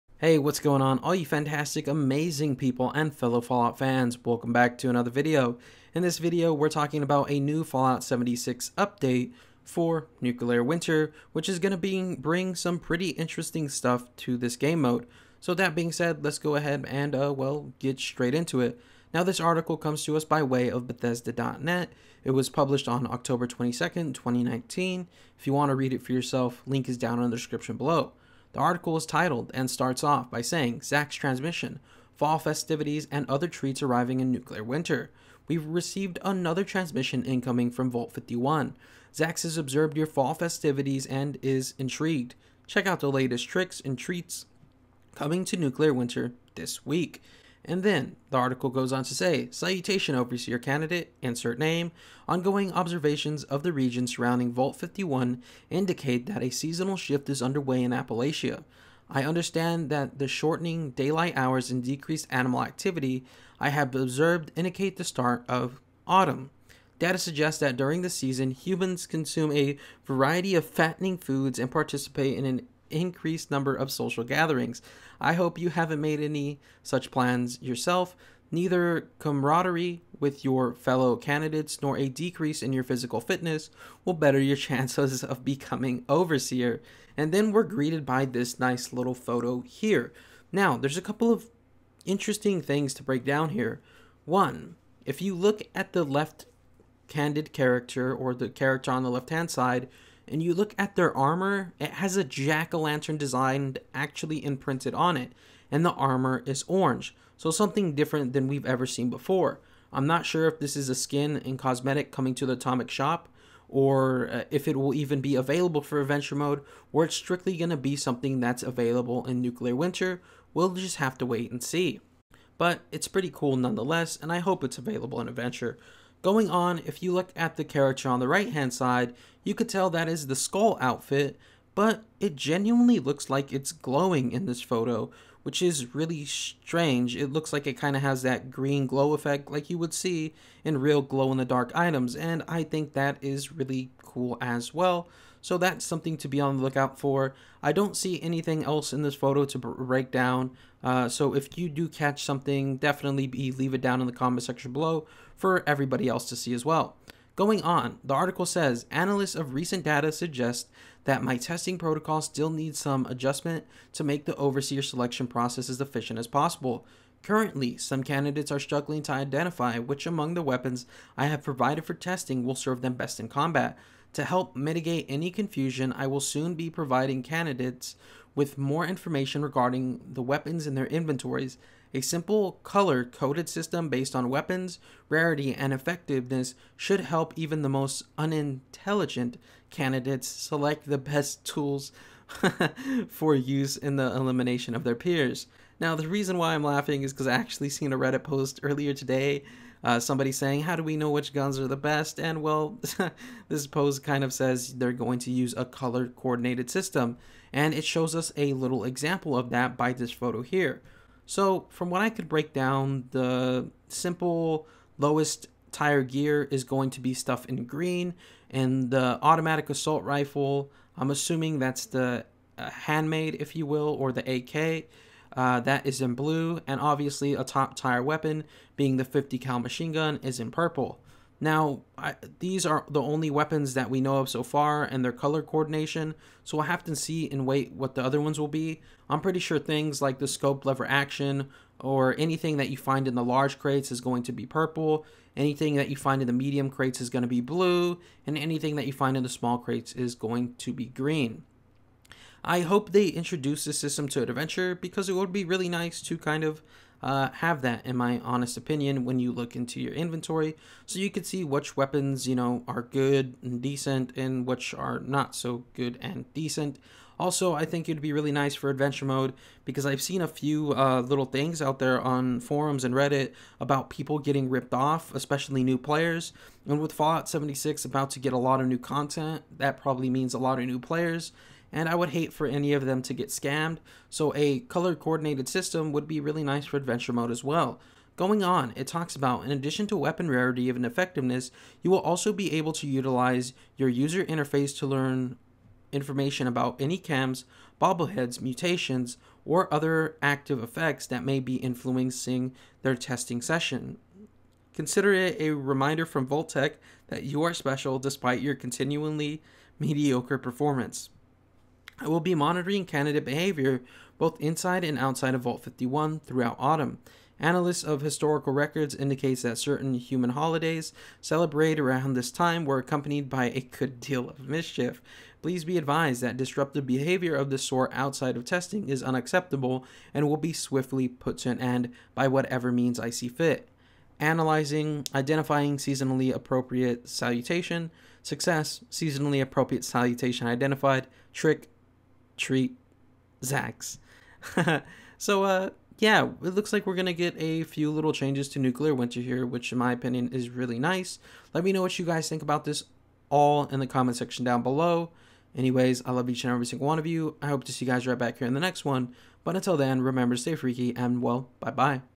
*New* Fallout 76 Nuclear Winter Update! | New Weapon System, Fall Festivities And Other Treats!
Hello and welcome to another video In this video I want to talk about the new update coming to nuclear winter. Feel free to criticize ideas, not people.

Article Used:
ZAX TRANSMISSION: FALL FESTIVITIES AND OTHER TREATS ARRIVING IN NUCLEAR WINTER: